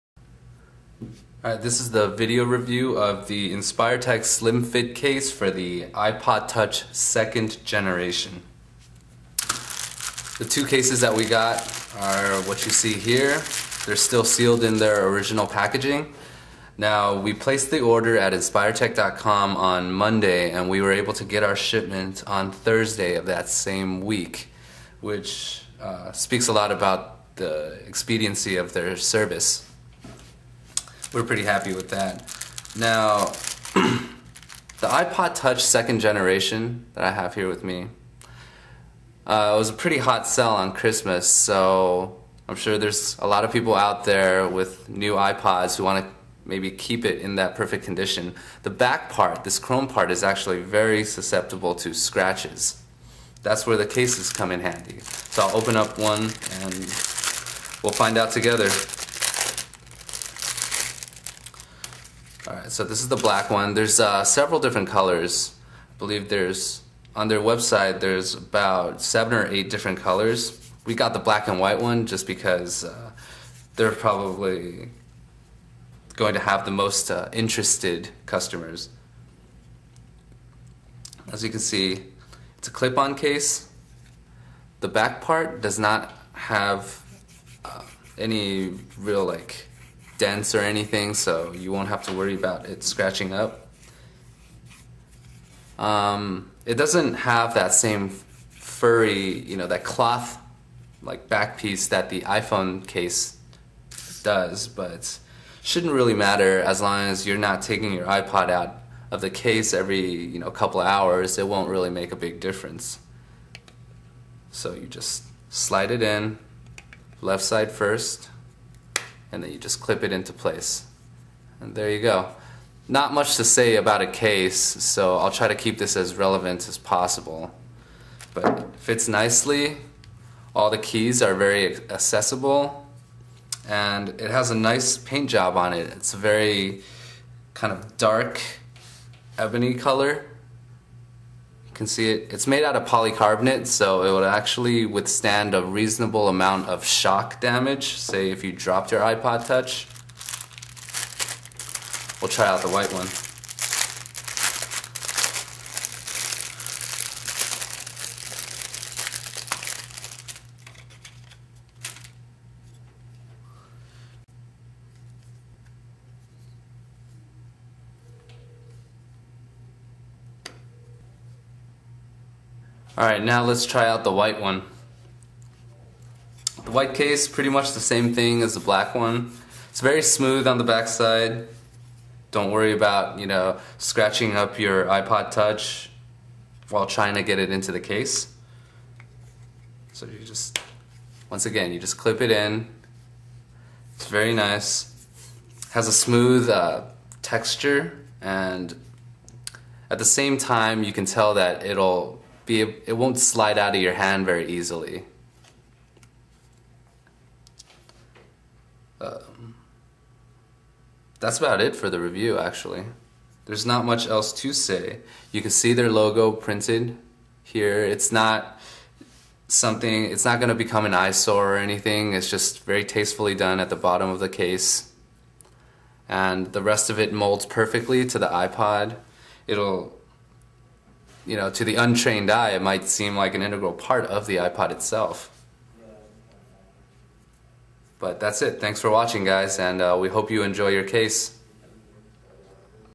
Review of iPod Touch 2nd Gen Case
Review of the new iPod Touch 2nd Gen. Case protectors from inspiretech. For the rest of the article, you can find us